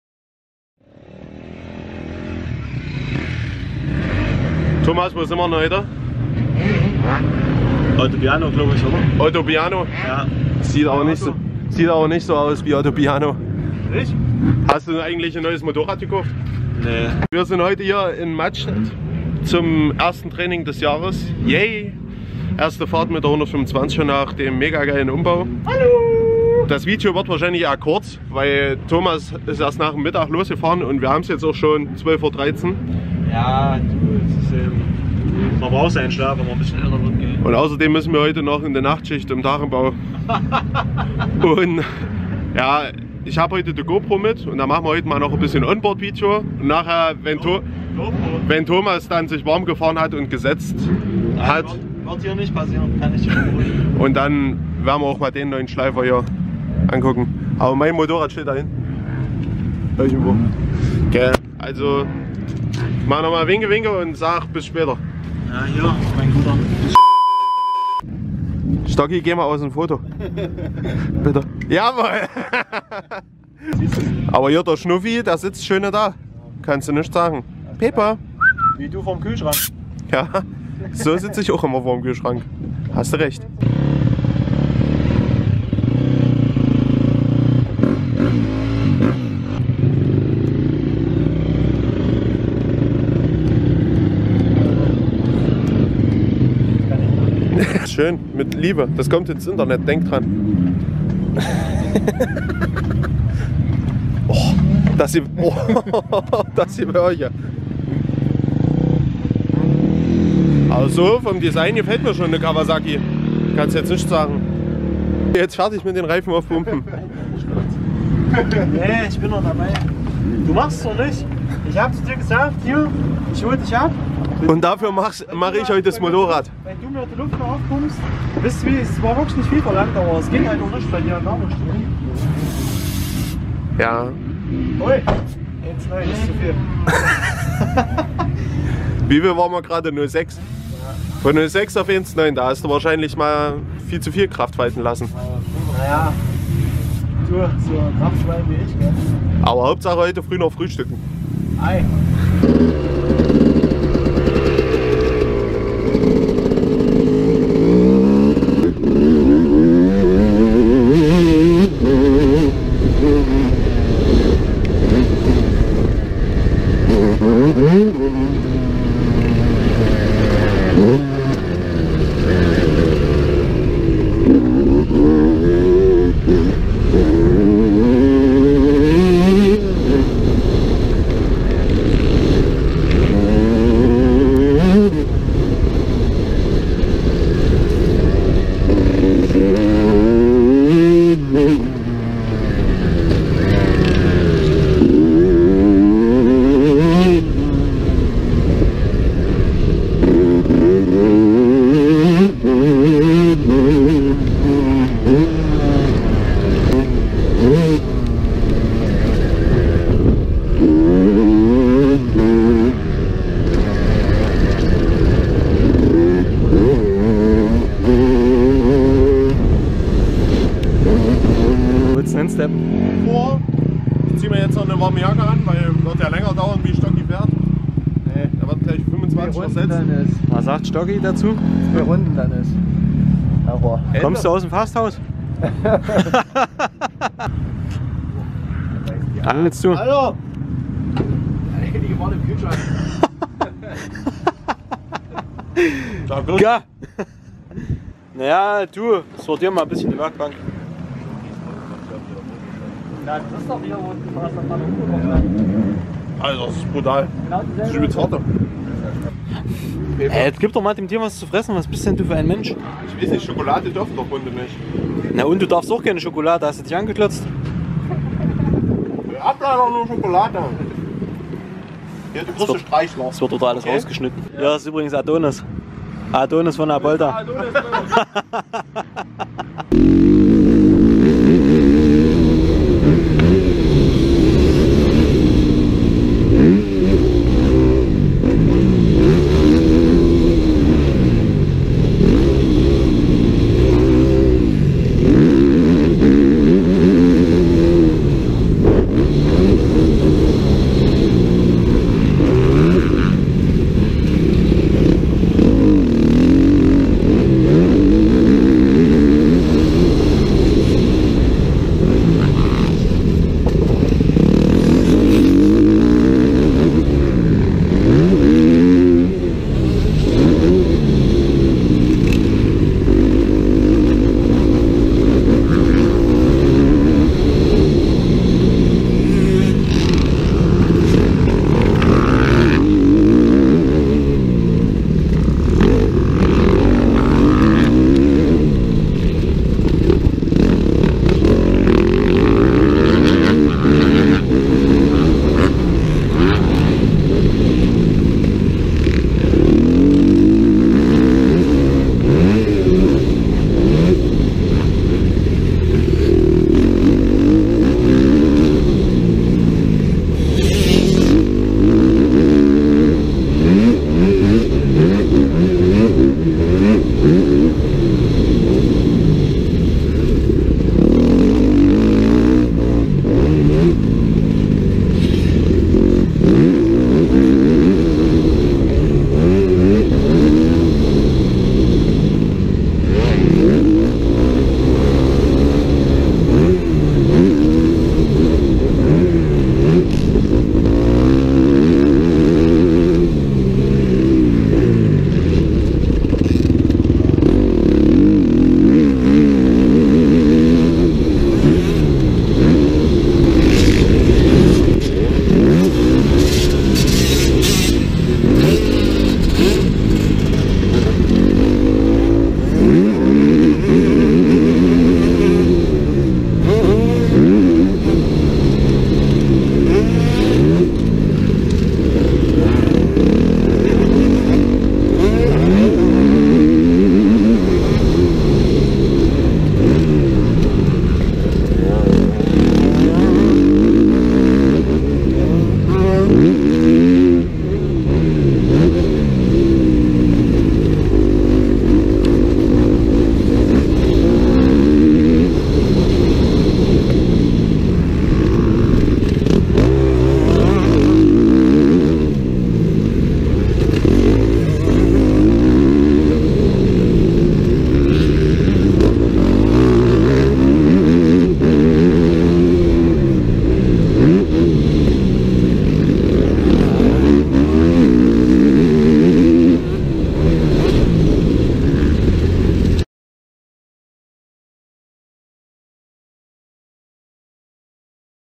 First MX Training 2021 | Mattstedt - Olister 94
Erstes Training des Jahres beim MSC Mattstedt

Liken - Teilen - Kommentieren